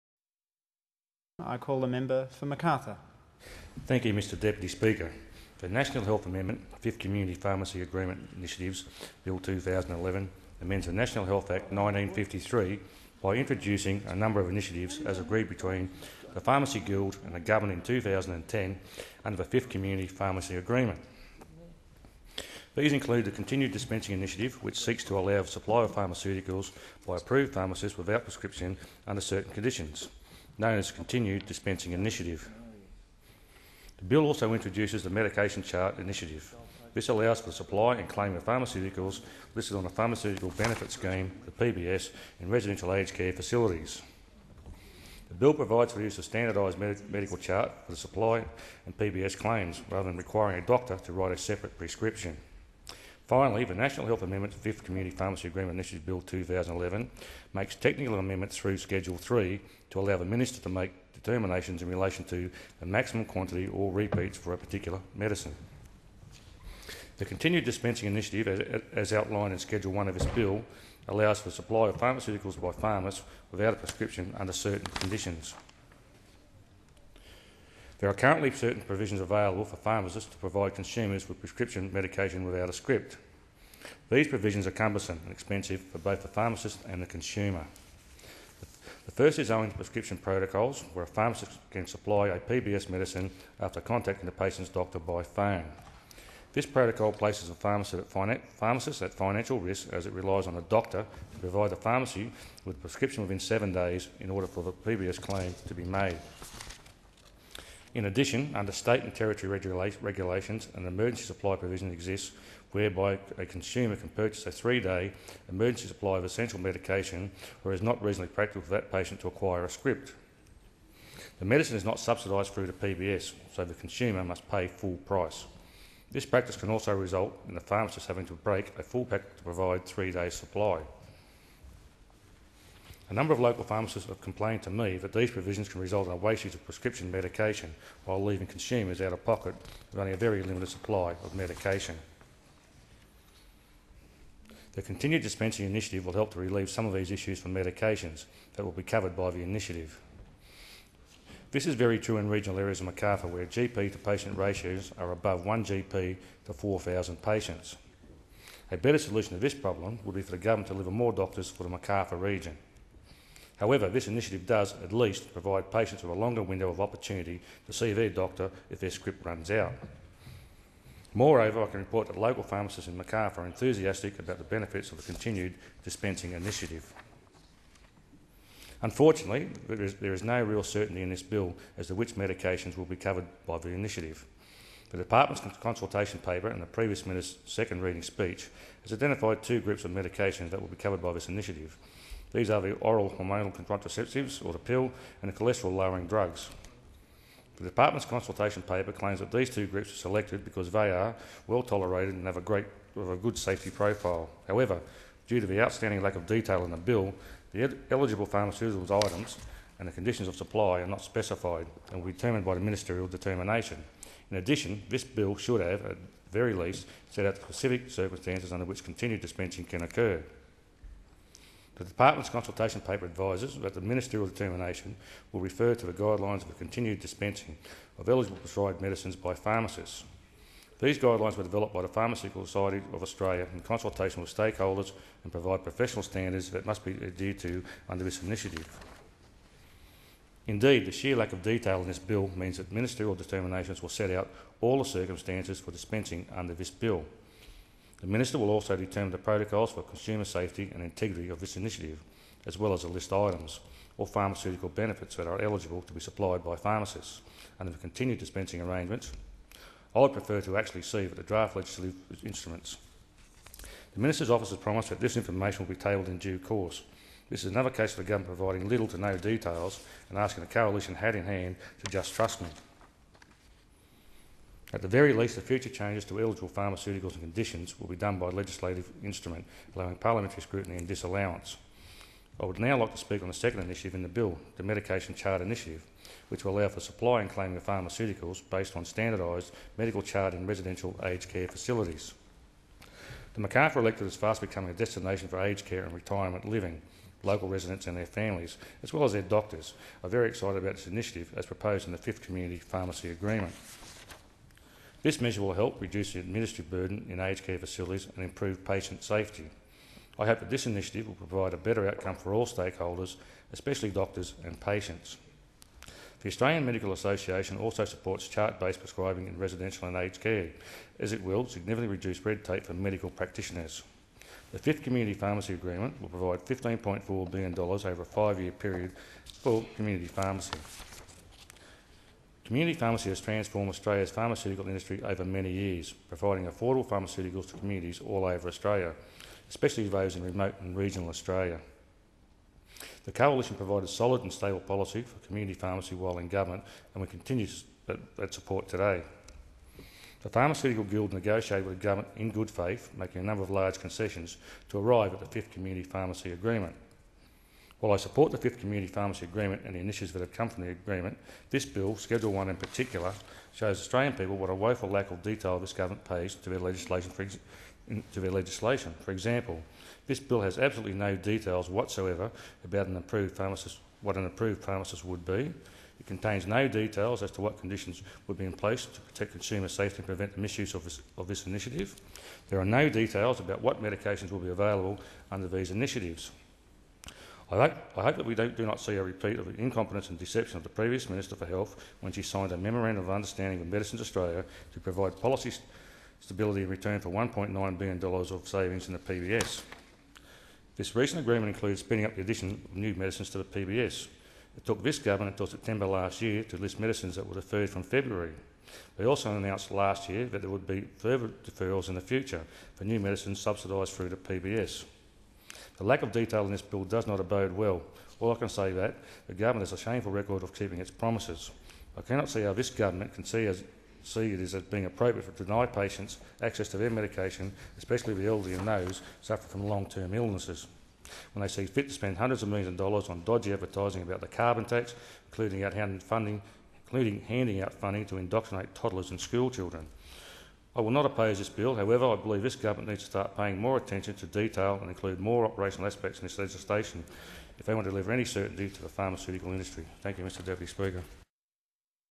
Pharmacy Agreement 16.2.12
Russell Matheson speaks about the National Health Amendment (Fifth community pharmacy agreement initiatives) Bill in parliament.